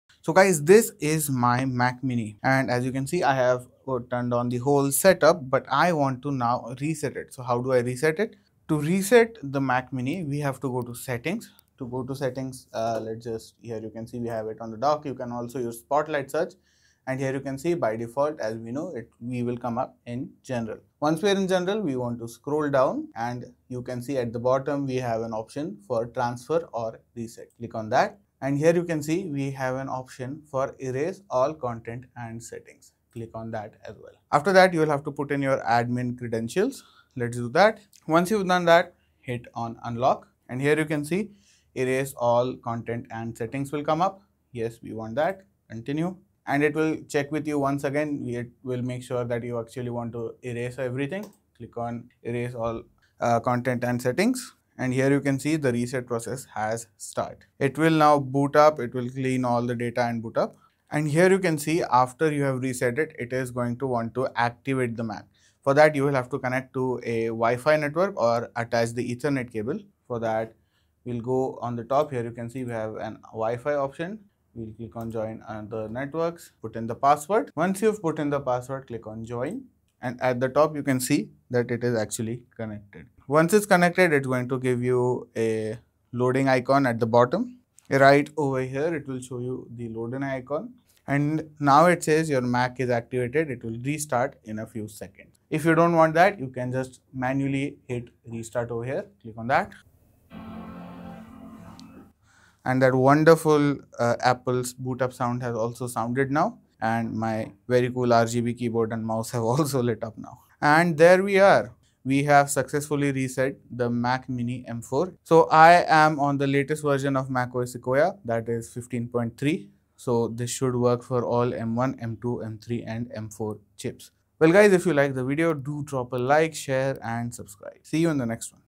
How To Reset Mac Mini? Resetting A Mac Mini Tutorial
This is a Mac Mini Reset tutorial. If you want to factory reset your Mac Mini, follow the simple steps given in this video.

If you want to erase all the personal data from your Mac Mini completely, you will have to Master reset your device. Resetting your Mac Mini is a simple and quick process. I have shown a step by step process of how to do a factory reset on a Mac Mini.